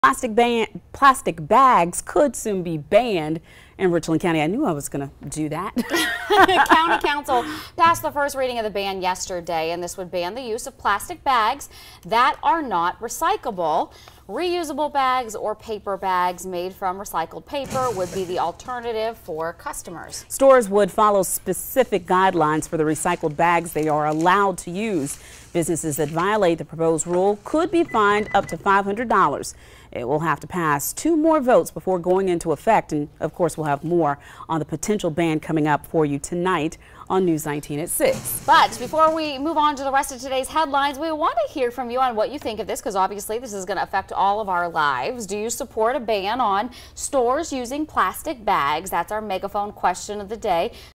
Possible plastic bag ban in Richland county
County council passed the first reading on Tuesday that would ban the use of plastic bags that are not recyclable.